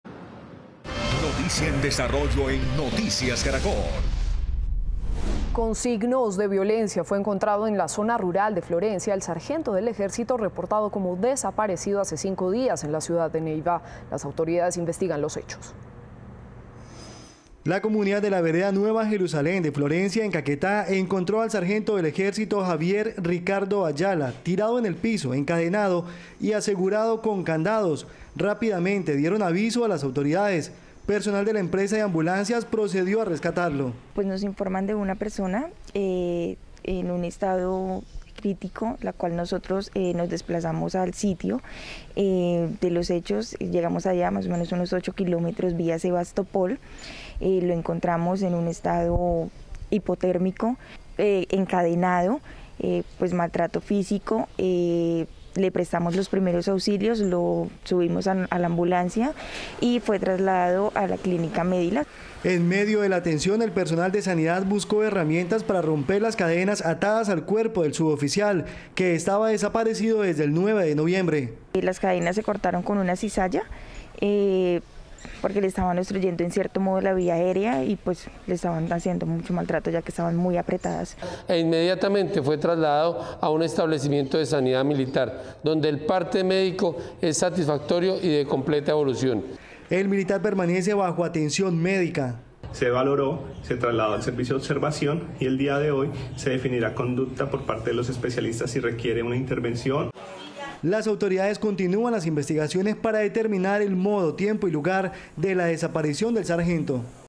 Encadenado y con signos de violencia, encuentran a sargento del Ejército desaparecido en Neiva
Con signos de violencia fue hallado en zona rural de Florencia el sargento del Ejército Javier Ricardo Ayala, reportado como desaparecido hace cinco días en la ciudad de Neiva.

La comunidad de la vereda Nueva Jerusalén halló al militar tirado en el piso, encadenado y asegurado con candados, además presentaba signos de violencia.

Rápidamente, avisaron a las autoridades y personal de la empresa de ambulancias procedió a rescatarlo.